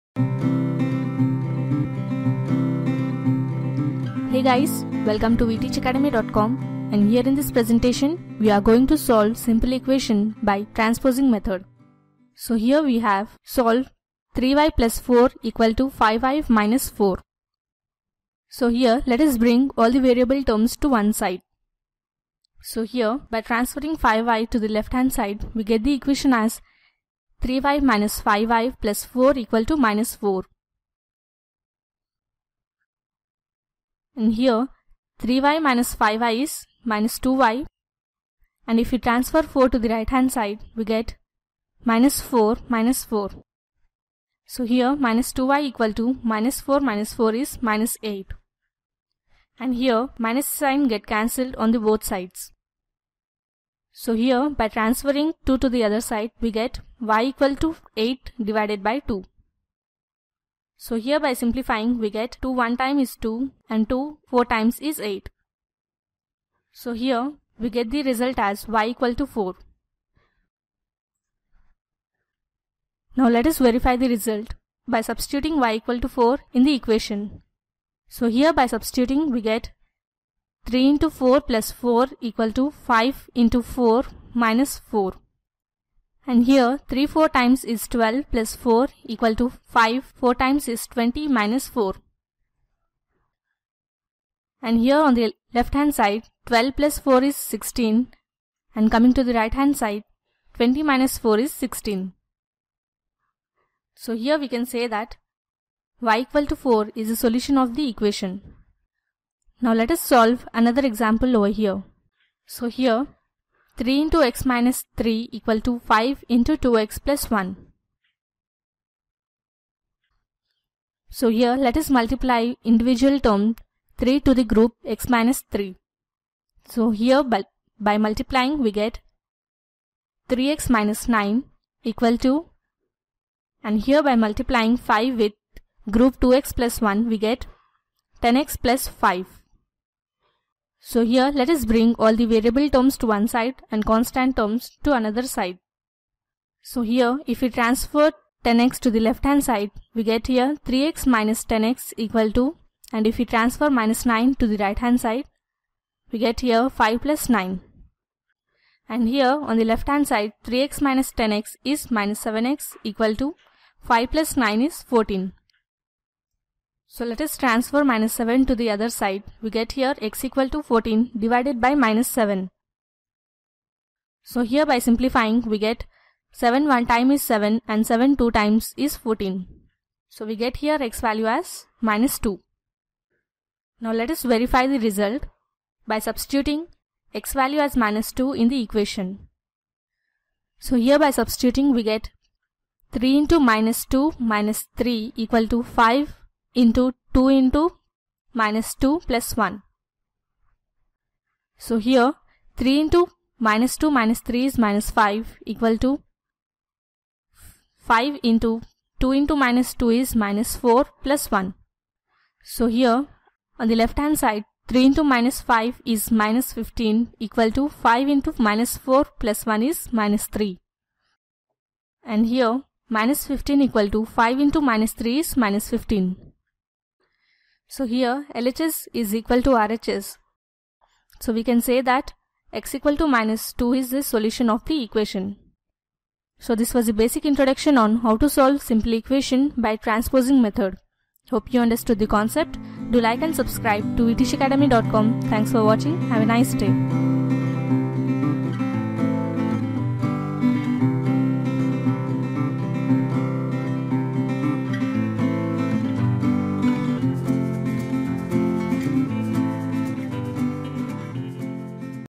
Solve Simple Equations By Transpose Method - Maths Algebra
Let us solve few examples of simple equations using Transpose method.